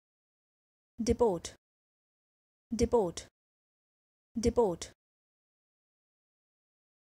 How to Pronounce Depot
Learn how to pronounce Depot correctly. This video shows you how to pronounce Depot correctly in American English.